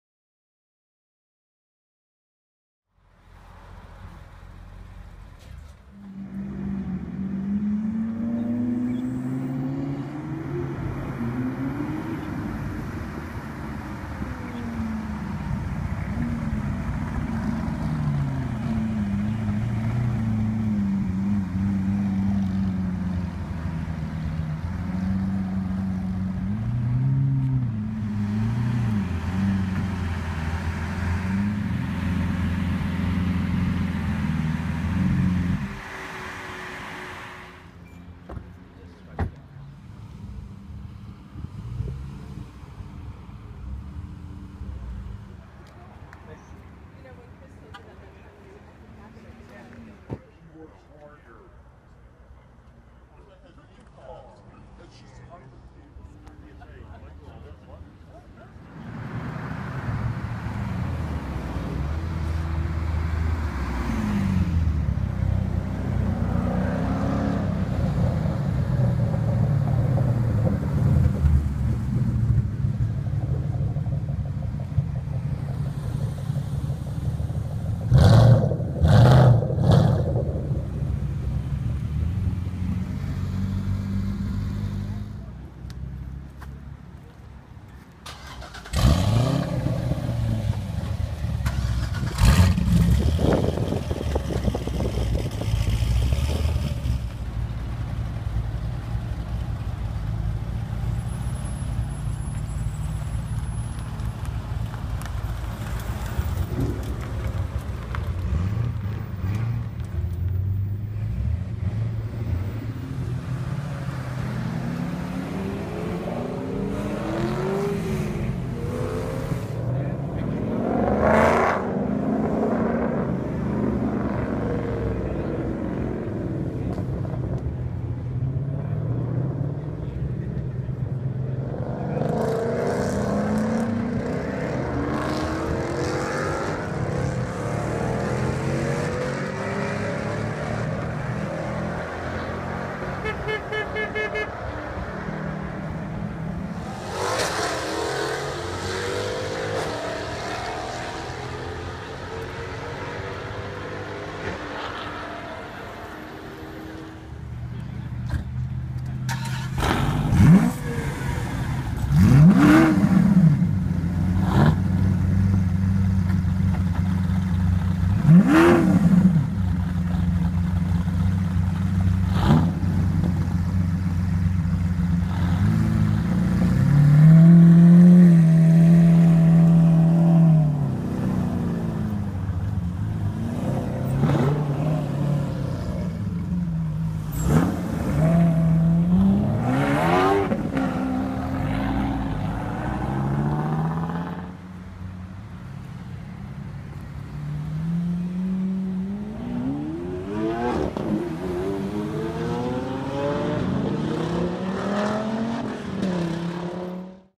Yorktown Cars & Coffee - 7/27/14
Here is the Yorktown Cars & Coffee event from 7/27/14. Enjoy!